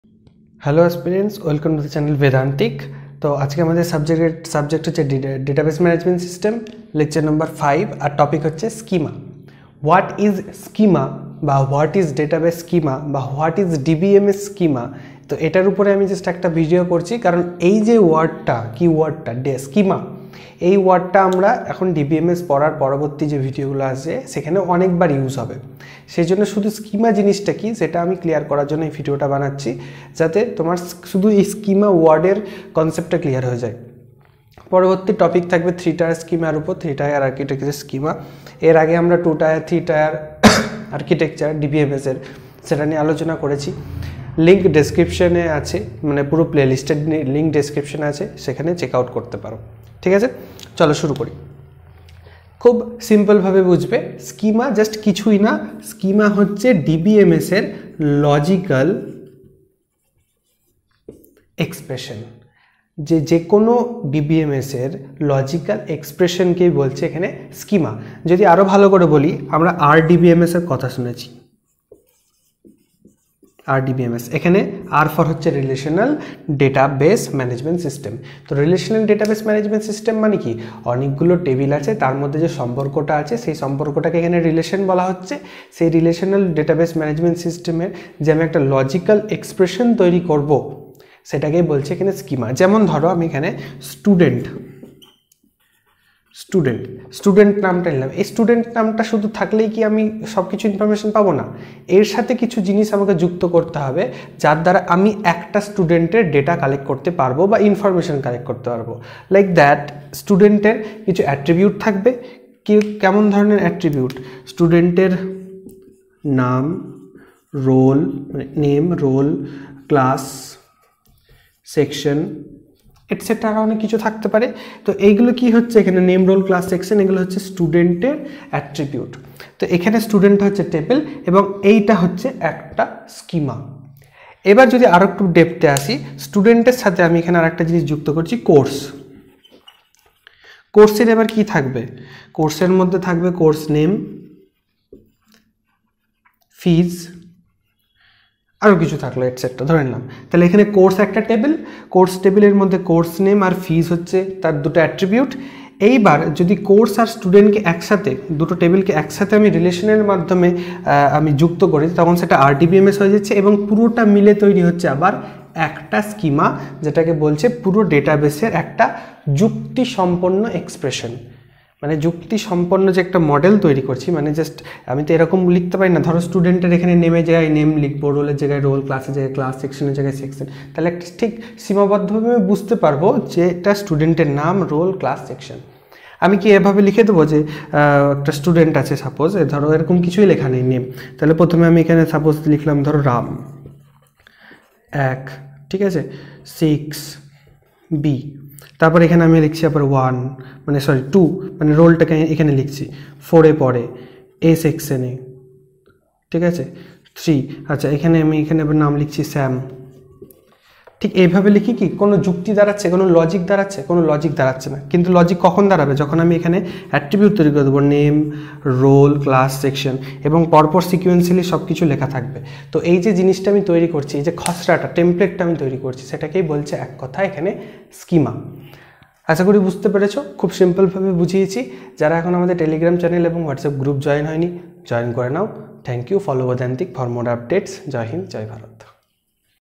What is Schema ? Database Schema | DBMS | Lec 5
Schema in DBMS refers to the logical and structural framework that defines the organization, storage, and retrieval of data within a database system. A schema outlines the relationships between data elements, defines data types and constraints, and provides a blueprint for data management. Understanding schema is crucial for effective database design and management. In this article, we explore the concept of schema in DBMS, its types, and how it affects database operations. Learn more about schema in DBMS and its significance in database management.